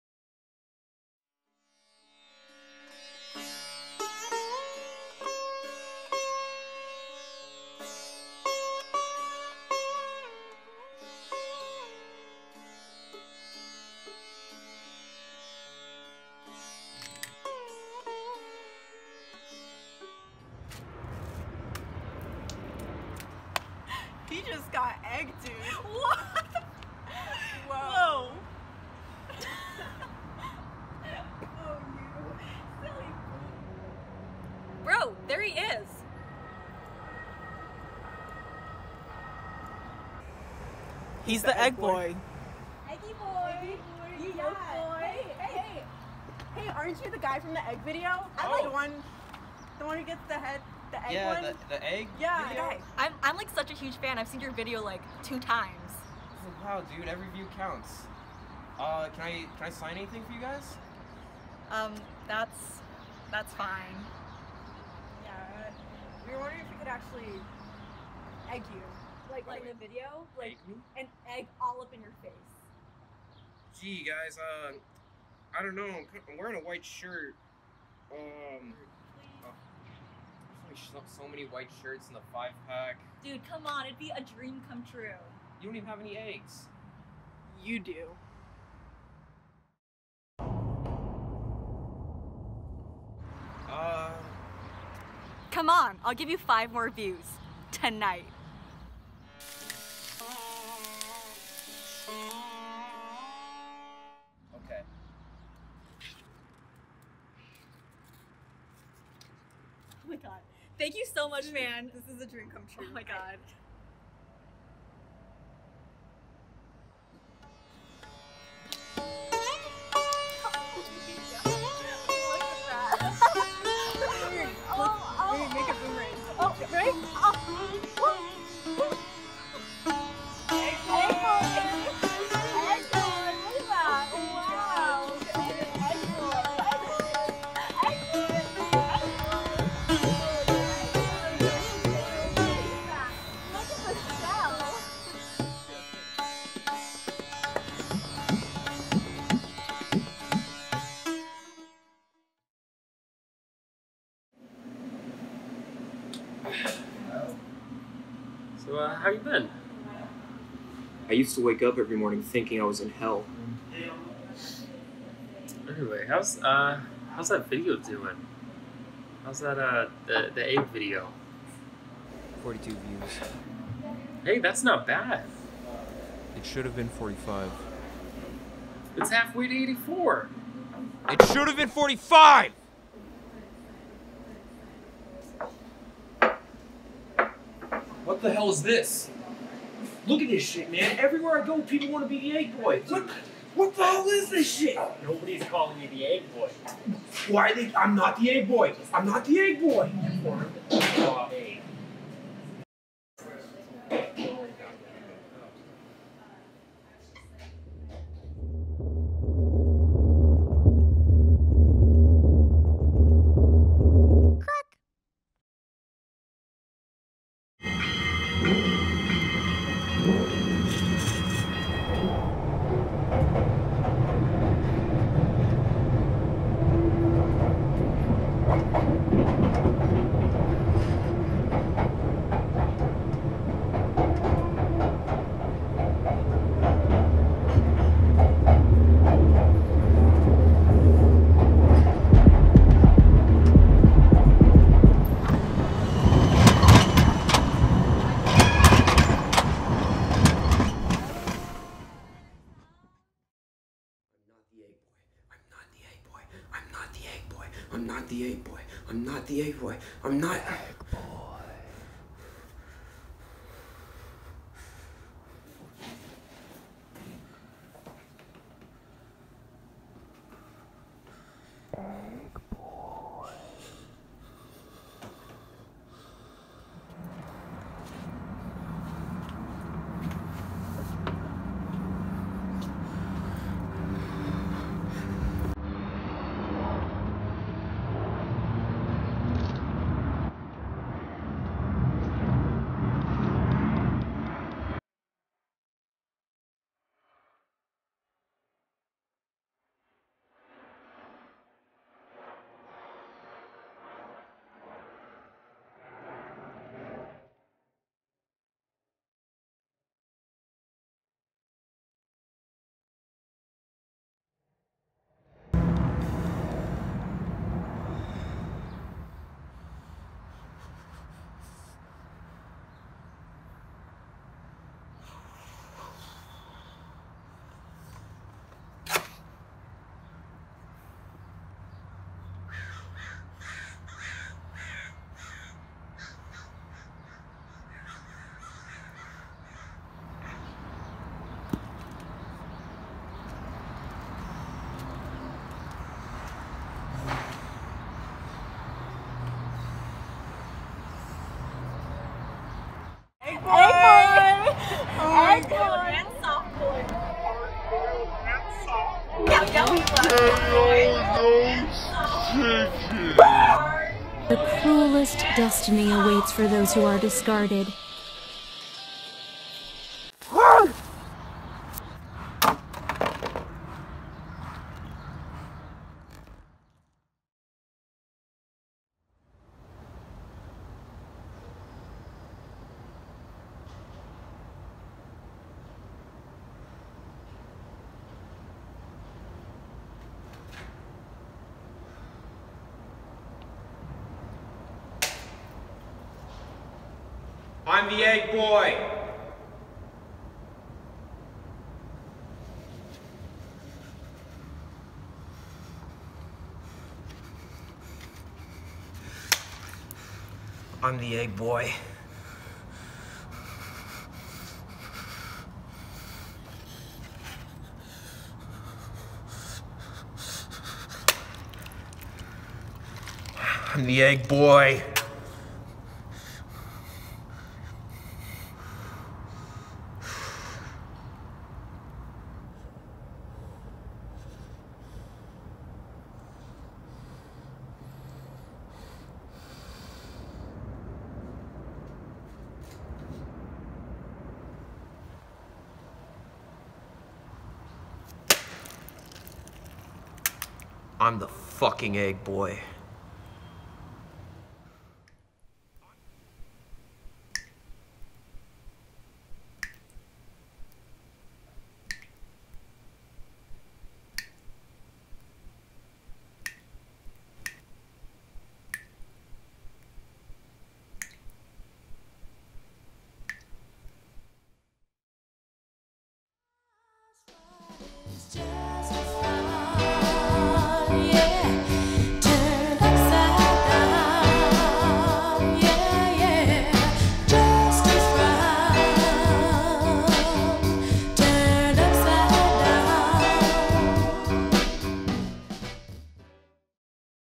Egg Boy
The cruelest destiny awaits those who are discarded.

Inspired by the short film "Egg Movie"